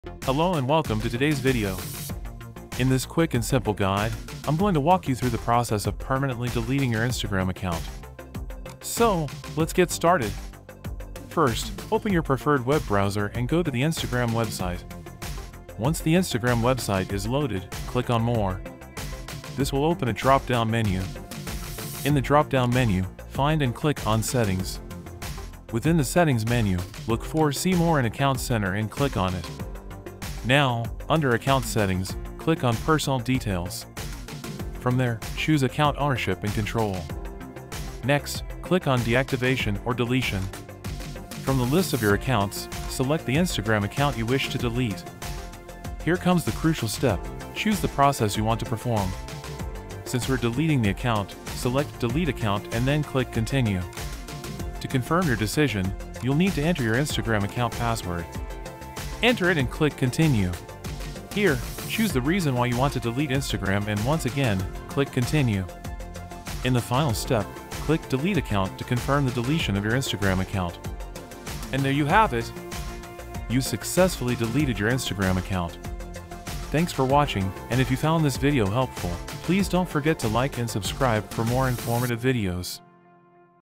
How to Delete Instagram Account: Quick and Simple Guide ( PC & Laptop)
Are you ready to bid farewell to Instagram? In this quick and simple guide, we'll show you how to delete your Instagram account using a PC or laptop. Whether you're taking a break or making a permanent exit, we've got your back.

DE
So löschen Sie Ihr Instagram-Konto: Schneller und einfacher Leitfaden (PC & Laptop)

Sind Sie bereit, sich von Instagram zu verabschieden? In diesem schnellen und einfachen Leitfaden zeigen wir Ihnen, wie Sie Ihr Instagram-Konto mithilfe eines PCs oder Laptops löschen können. Egal, ob Sie eine Pause einlegen oder einen dauerhaften Abschied planen, wir haben Sie abgedeckt.